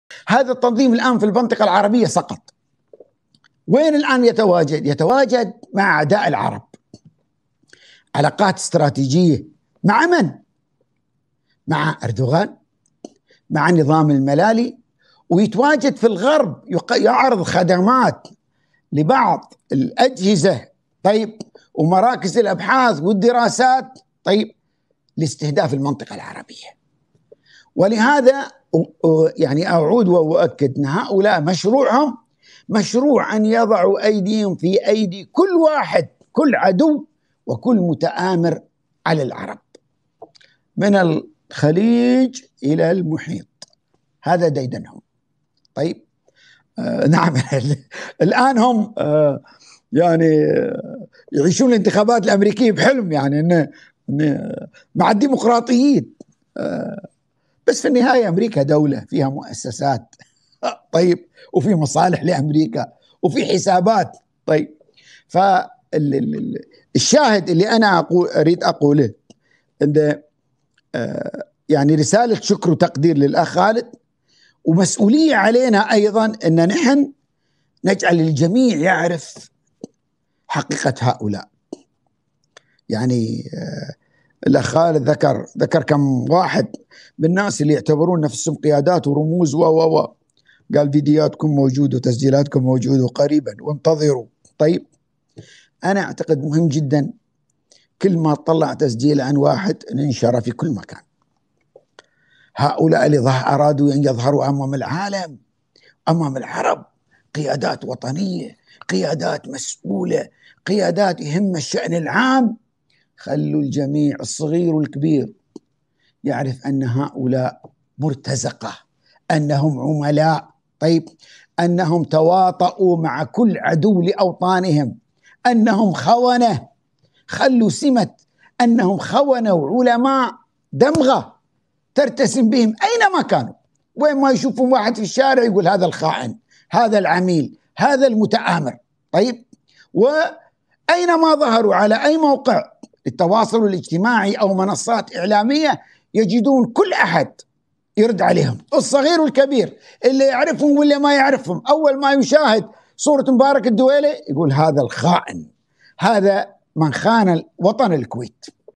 د. علي النعيمي: الإخوان سقطوا وانتهوا من المنطقة العربية وحاليا يتواجدون بالدول المعادية لنا
د. علي_النعيمي: الإخوان سقطوا وانتهوا من المنطقة العربية وحاليا يتواجدون بالدول المعادية لنا لاستهداف منطقتنا العربية

‎تابعوا فرسان_الإمارات عبر حساباتنا التالية